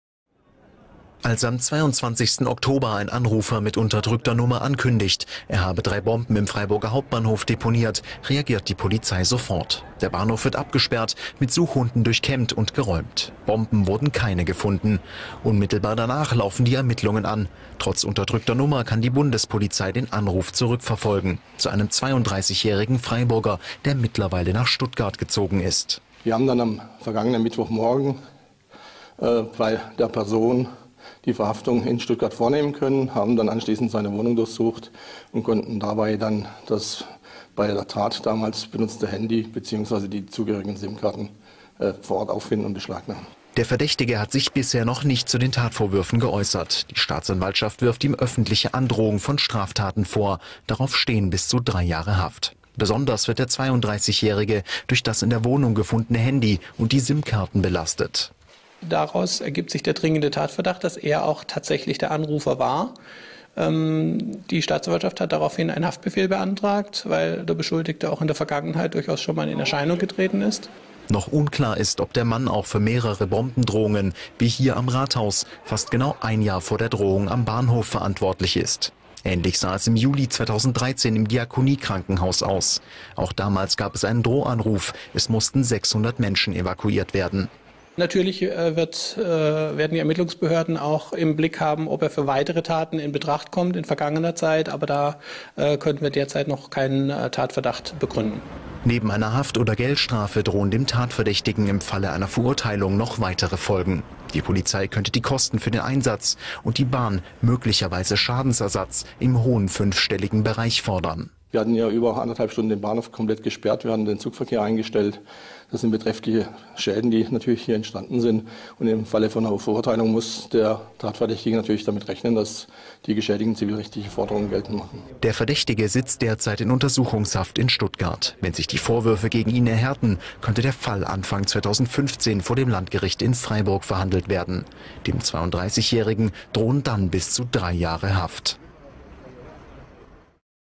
Freiburg - Polizei fasst mutmaßlichen Täter
Nachdem der Freiburger Hauptbahnhof Ende Oktober 2014 wegen einer Bombendrohung evakuiert werden musste, hat die Polizei nun den mutmaßlichen Täter gefunden.